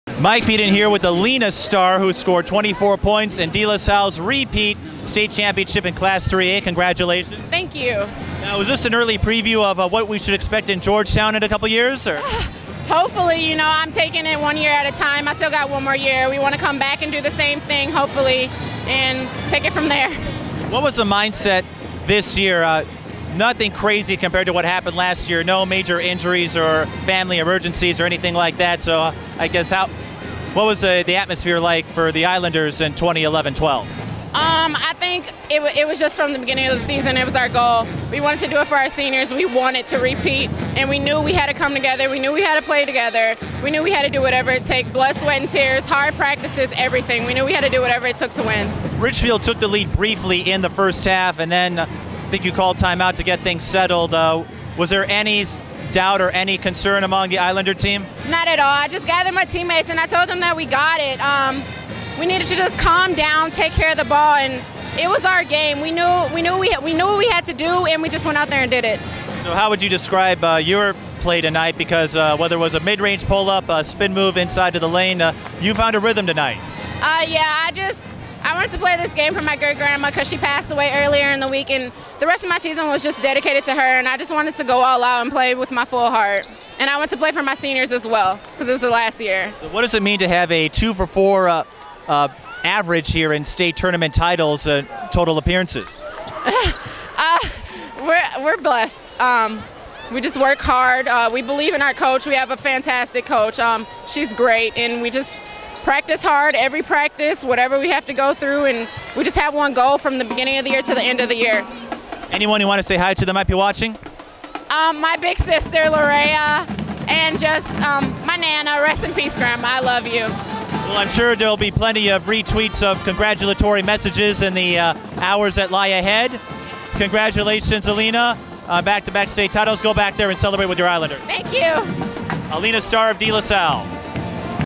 2012 state tournament: Allina Starr of DeLaSalle
DeLaSalle junior and future Georgetown Hoya Allina Starr talks with Mike Peden after scoring 24 points to defeat Richfield in the class AAA state tournament.